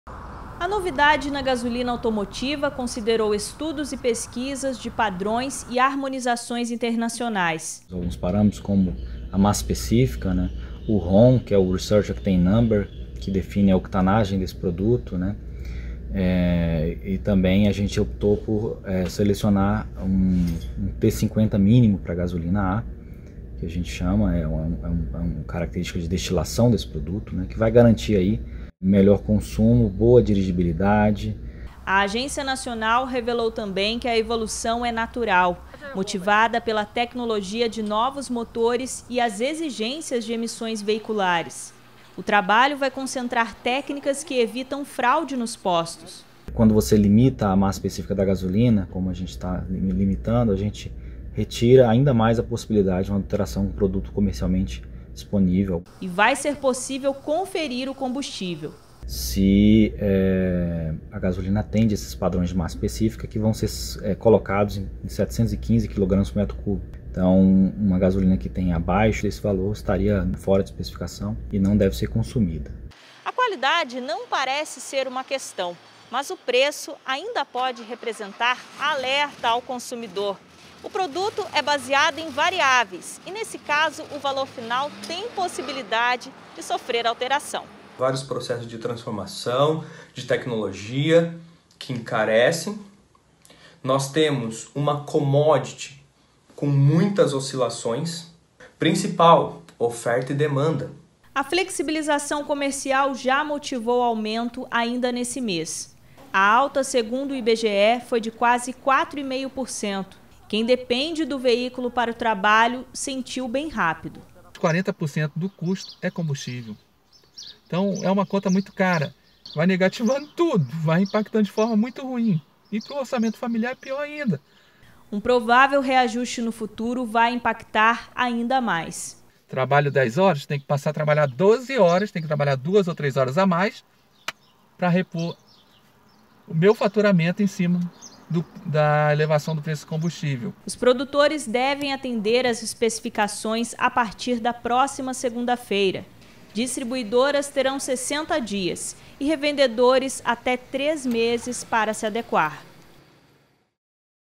Nova gasolina terá mais qualidade e provável aumento no preço
A partir do mês que vem a gasolina produzida no Brasil terá novas especificações. A modificação é um meio de proporcionar maior qualidade, eficiência energética e contribuições ao meio ambiente. Apesar do preço não estar em questão, o aumento nas bombas não é descartado.

Reportagem de Fernanda Ribeiro e Genilson Pacetti